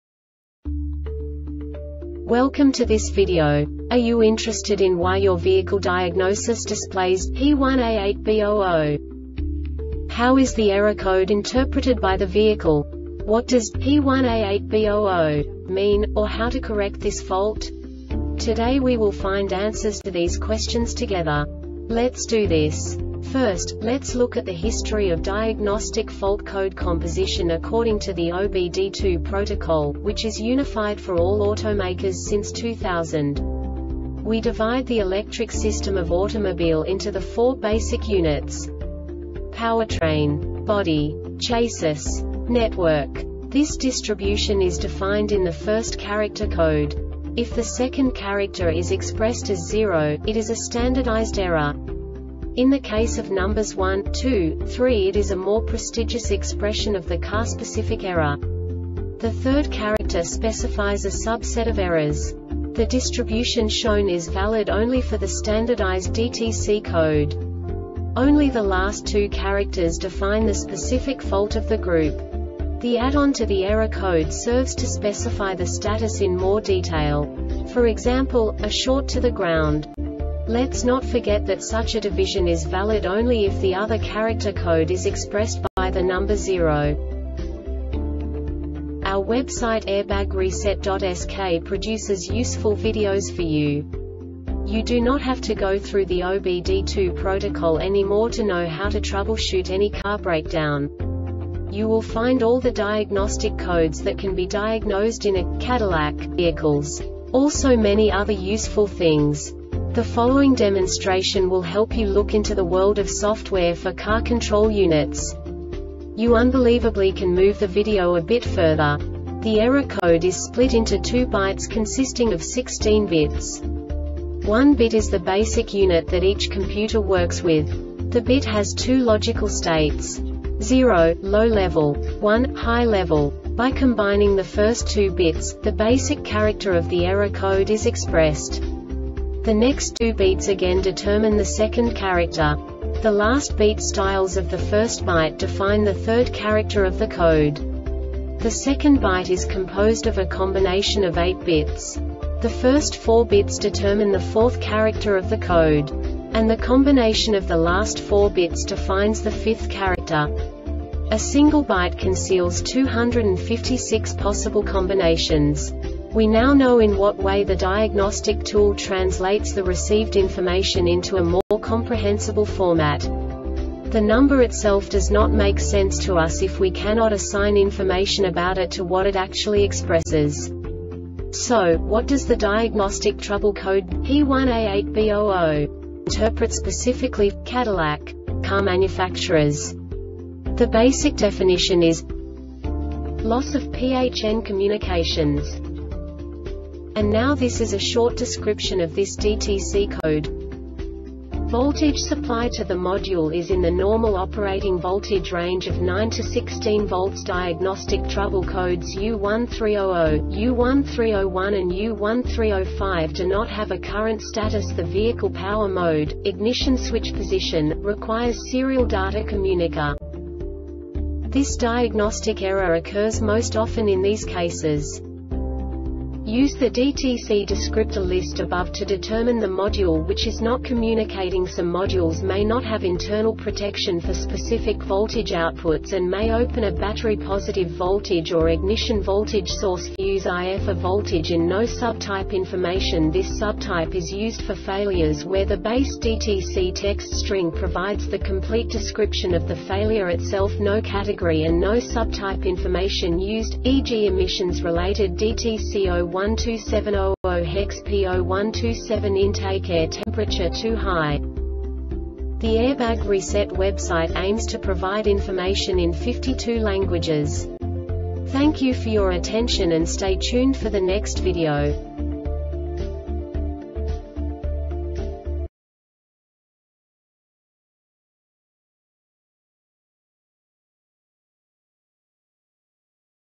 DTC cadillac P1A8B-00 Short Explanation
Contents:
0:21 Basic DTC analysis according to OBD2 protocol standard.
1:48 Insight into programming
2:58 A diagnostically understandable description of the Diagnostic Trouble Code
3:05 Basic error definition

Make:
Cadillac
Code:
P1A8B 00
Definition:
Loss of PHN Communications
Description: